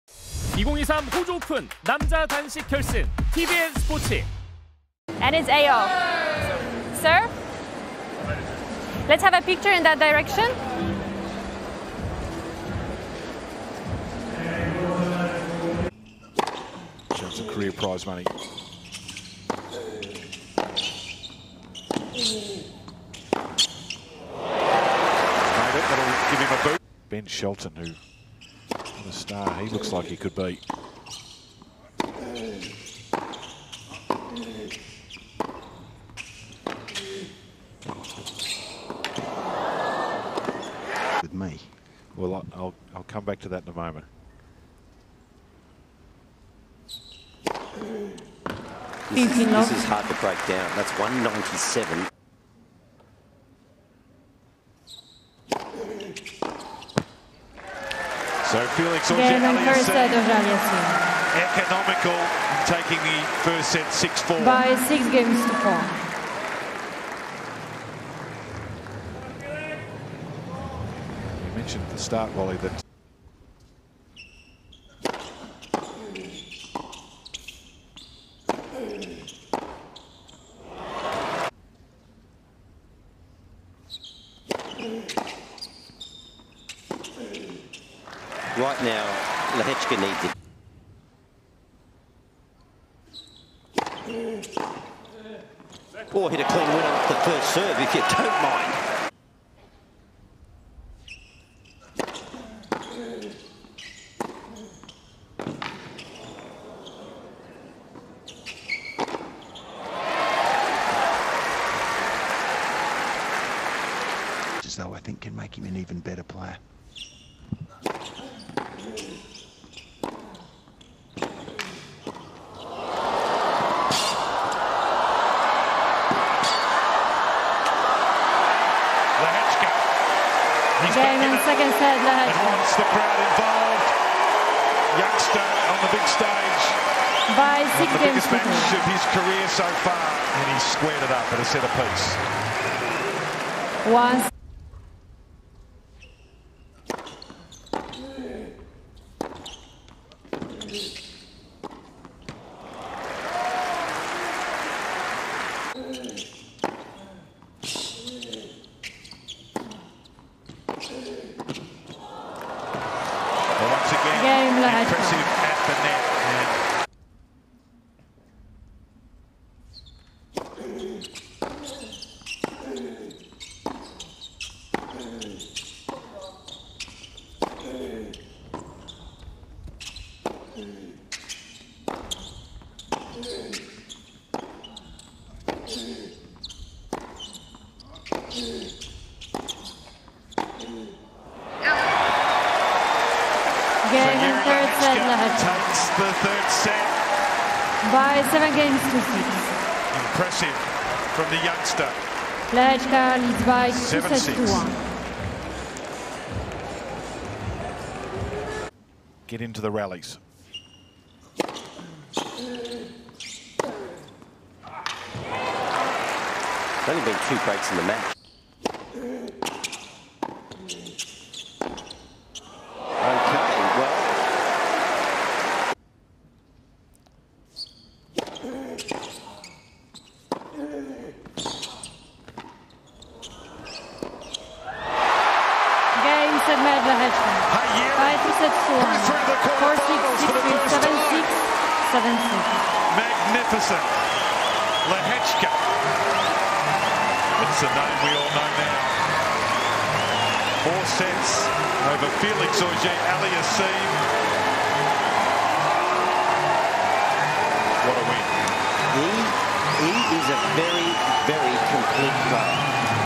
[2023 호주 오픈] Day-7 본선 4라운드 남자단식 이르지 레헤츠카 vs 펠릭스 오제-알리아심
본선 4라운드 남자단식 이르지 레헤츠카 vs 펠릭스 오제-알리아심(Jiri LEHECKA vs Felix AUGER-ALIASSIME)